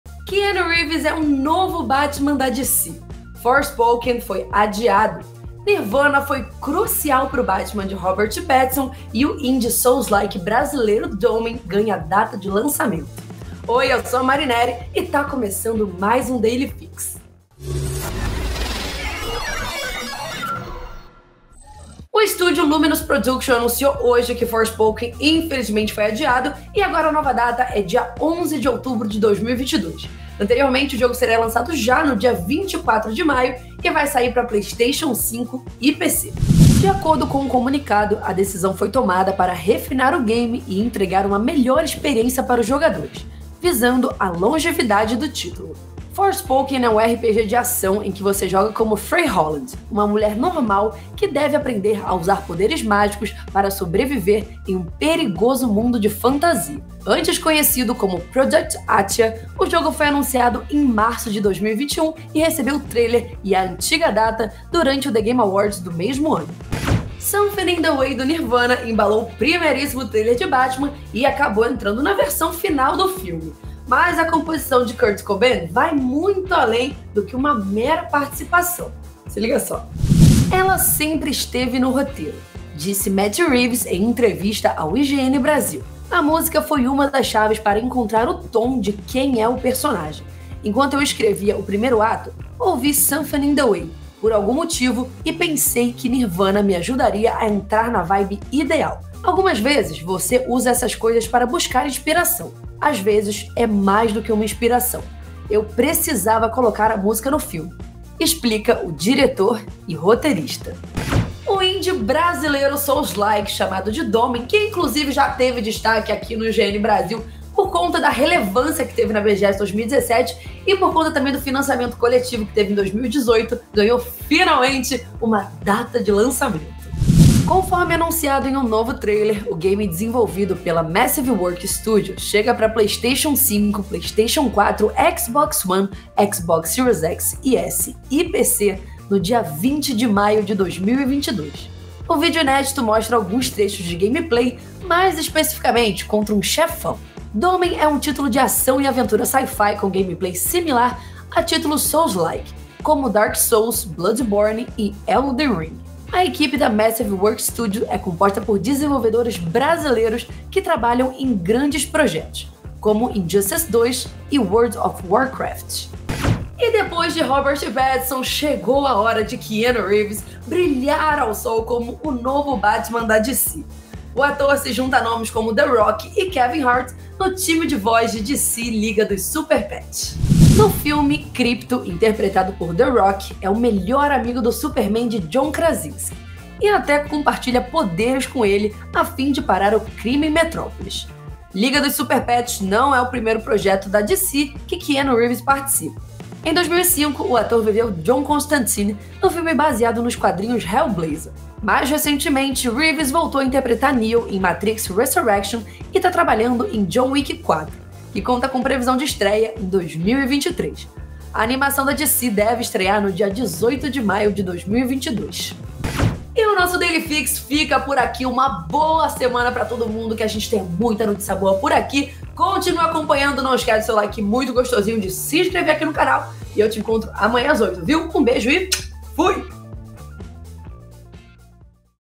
KEANU REEVES É NOVO BATMAN DA DC - Daily Fix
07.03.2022 - Forspoken é adiado; Nirvana foi crucial para o Batman de Robert Pattinson; Dolmen, souls-like brasileiro, ganha data; Keanu Reeves é o Batman da Liga dos Superpets.

ACOMPANHE O IGN BRASIL!